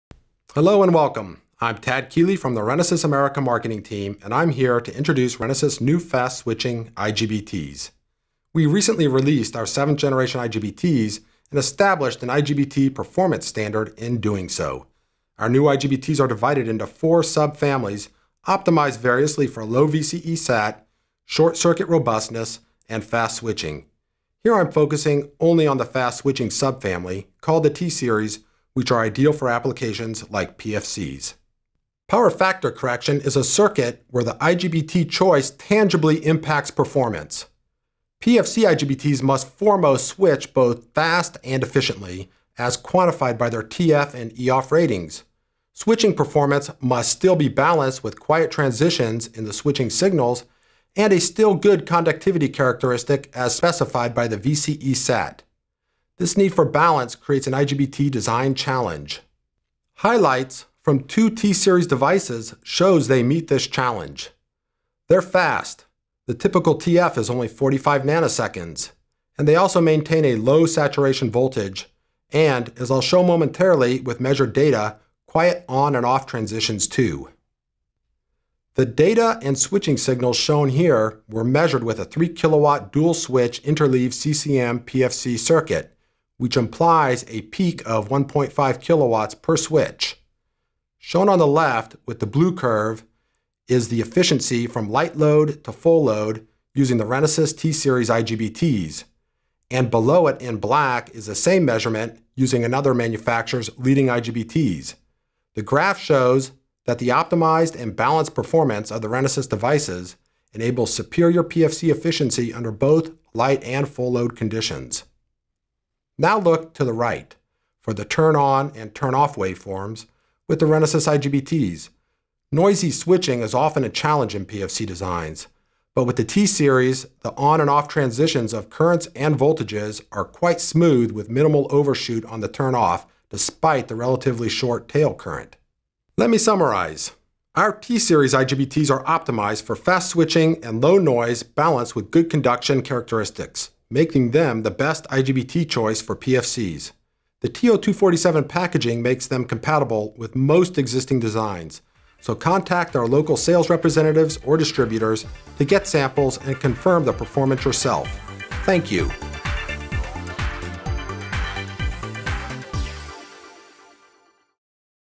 Renesas IGBTs for Power Factor Correction
Renesas T-Series IGBTs are optimized for performance in PFC’s and other circuits requiring fast switching.  The T-Series IGBTs come in industry standard To247 packages and can conduct up to 45amps when hot.

Contact Renesas via our distributors or local sales office to get a quote and samples.